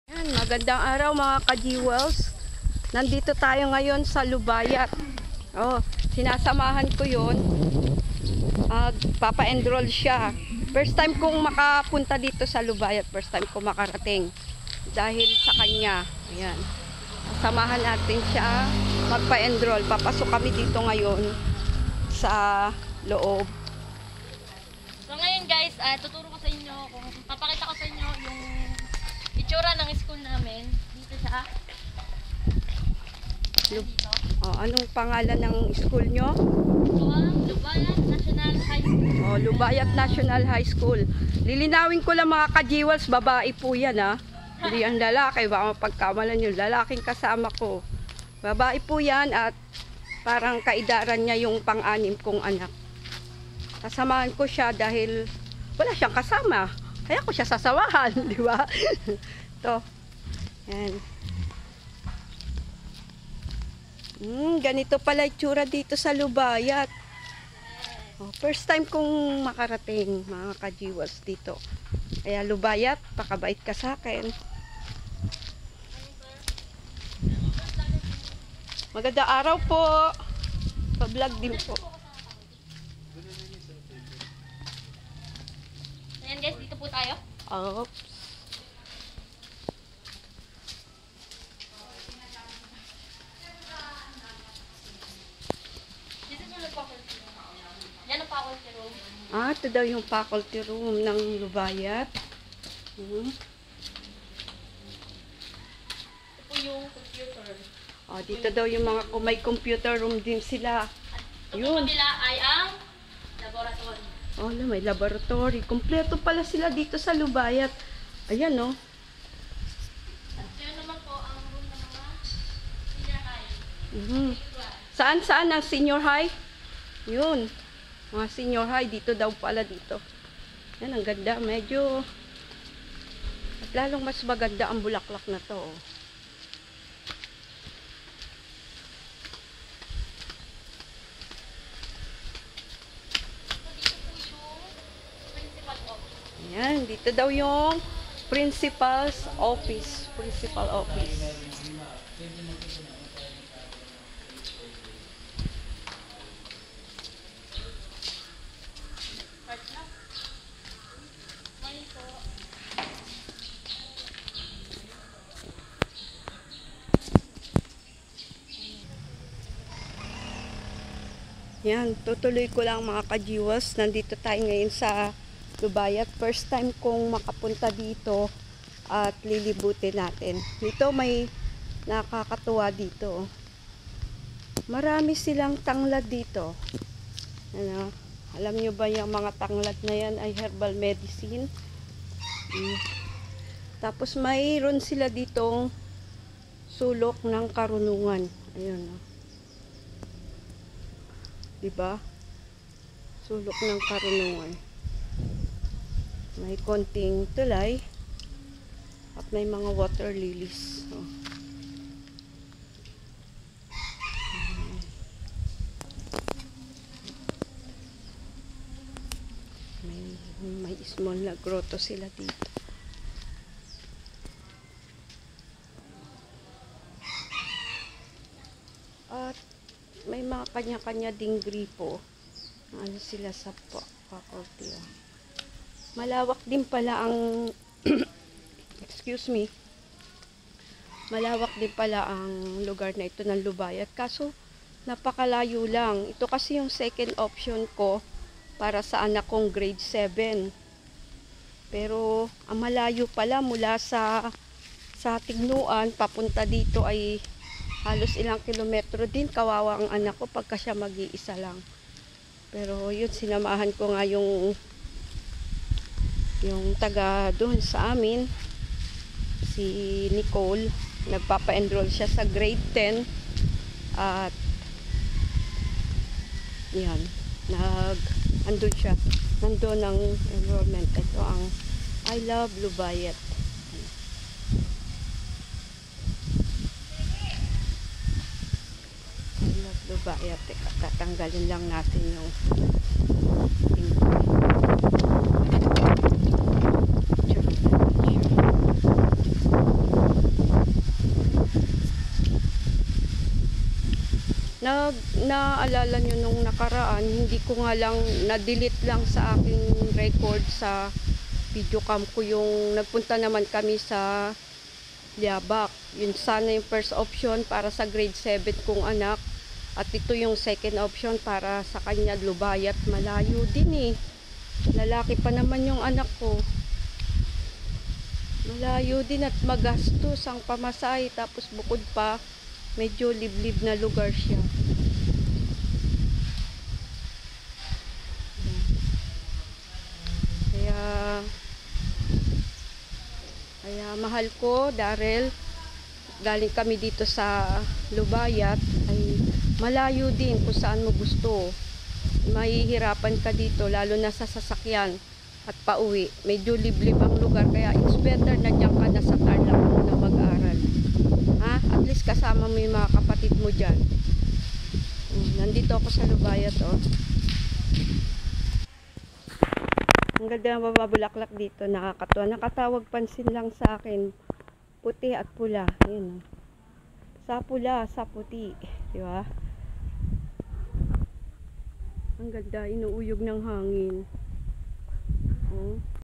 LUBAYAT NATIONAL HIGH SCHOOL [ ENROLLMENT | FIRST TIMER | SCHOOL BRIGADE ] SA WAKAS NARATING KO RIN😍
Ang tagal ko ng Plano na puntahan ang lugar sa hindi inaasahang pagkakataon narating ko din,😘😊😍